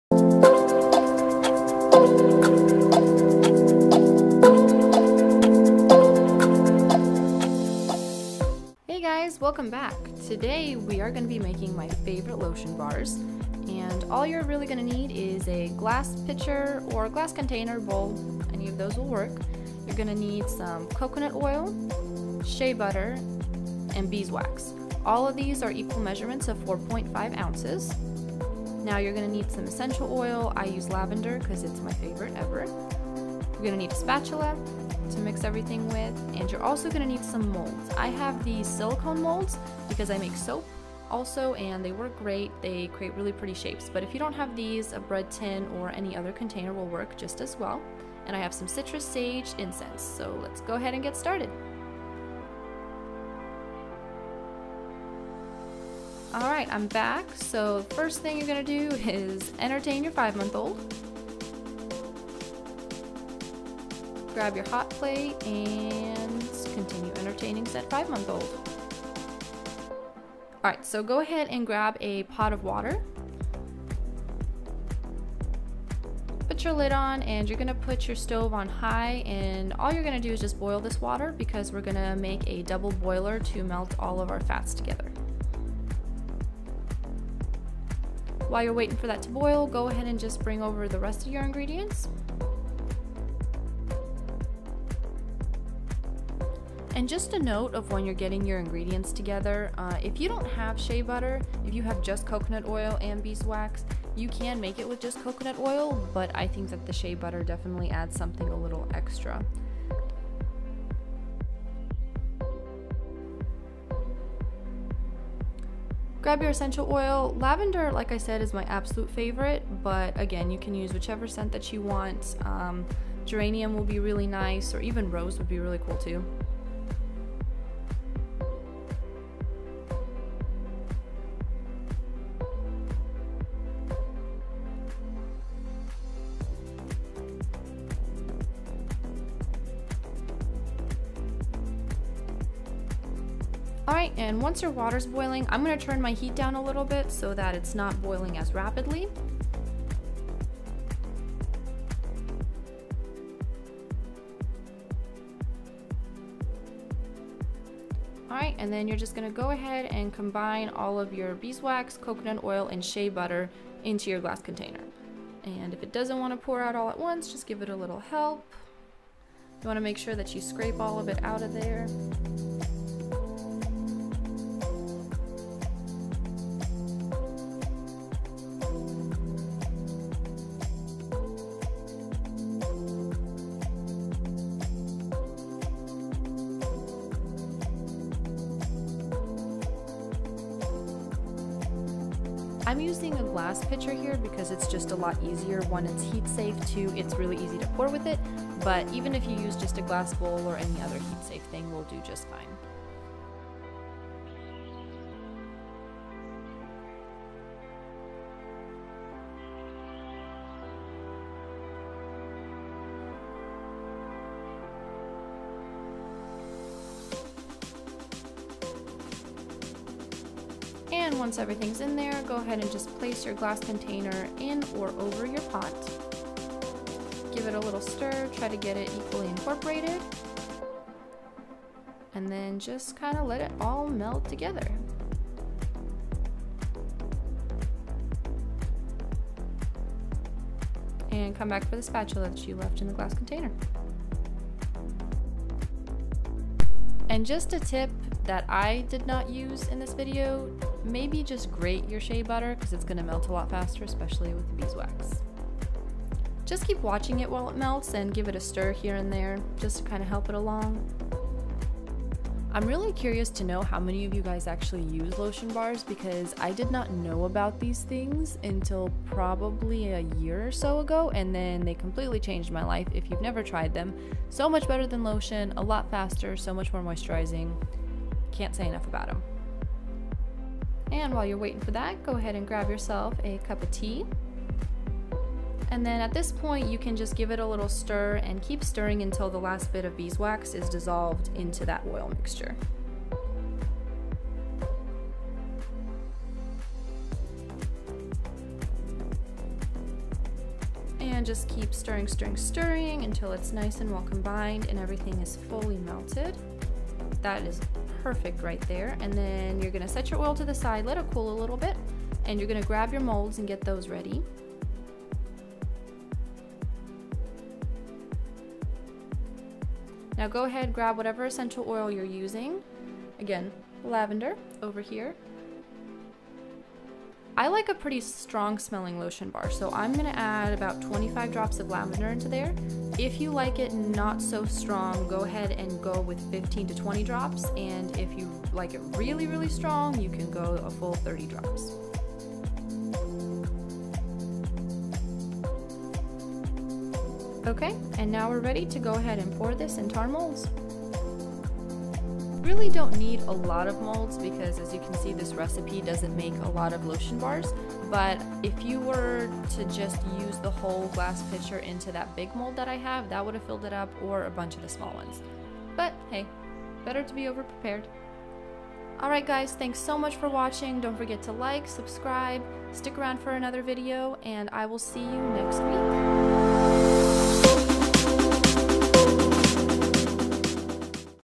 Shea Butter Lavender Lotion Bar DIY
Hello Everybody!
Hope everyone is staying safe and doing well during these times. Today we will be making an amazing lotion bar! Lets use all natural oils and butters to make our own lush lotion bar! You can mix and match different essential oils for your favorite scent. Enjoy!

Ingredients:
- 4.5 oz Beeswax
- 4.5 oz Coconut Oil
- 4.5 oz Shea Butter
- 20 - 30 Drops of Lavender Essential Oil (or fragrance of choice)